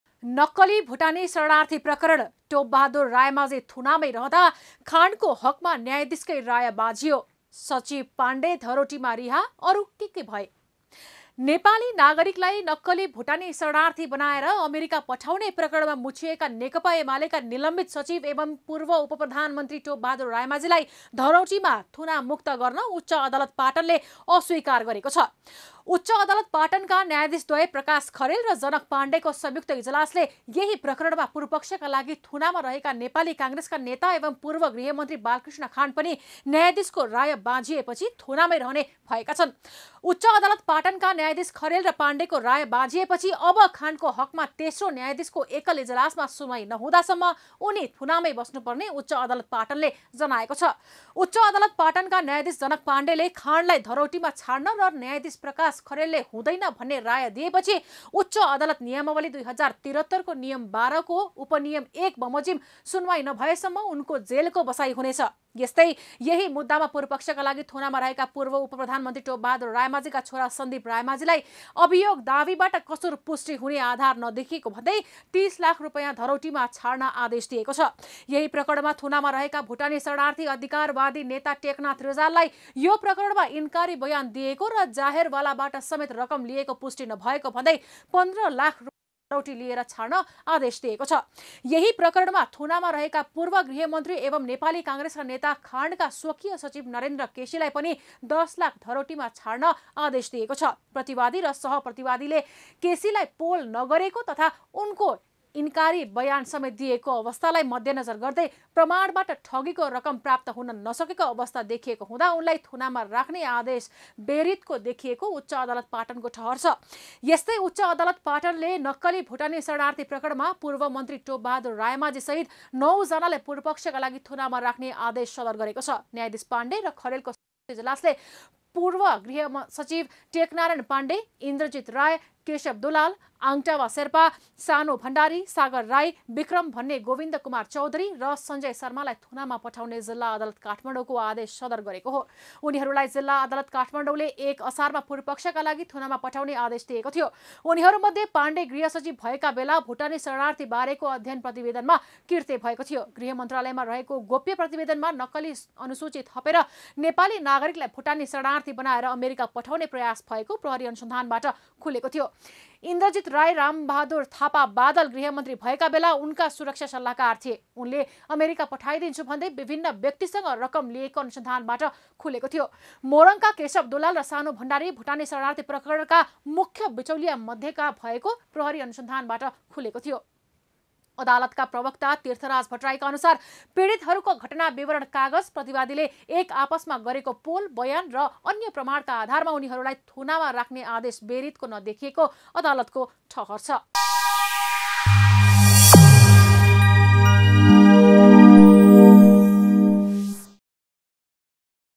टोपबहादुर रायमाझी थुनामै रहँदा, खाणको हकमा न्यायाधीश कै राय बाझियो,अरु के-के भए ?
नक्कली भुटानी शरणार्थी प्रकरण: टोपबहादुर रायमाझी थुनामै रहँदा, खाणको हकमा न्यायाधीश कै राय बाझियो, सचीव पाण्डे धरौटीमा रिहा, अरु के के भए ?